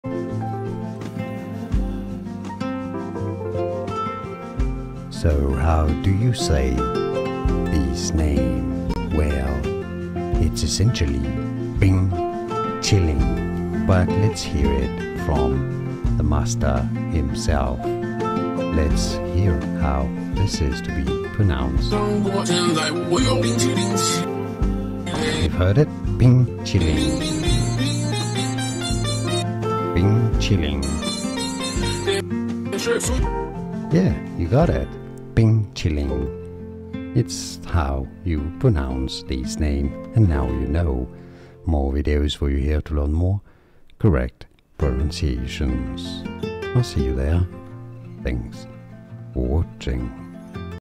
How to Pronounce 'Bing Chilling'
This video shows you how to pronounce Bing Chilling, John Cena, Chinese, pronunciation.

00:00 – Bing Chilling Intro
00:08 – Bing Chilling Pronunciation Guide
00:31 - More Difficult Pronunciations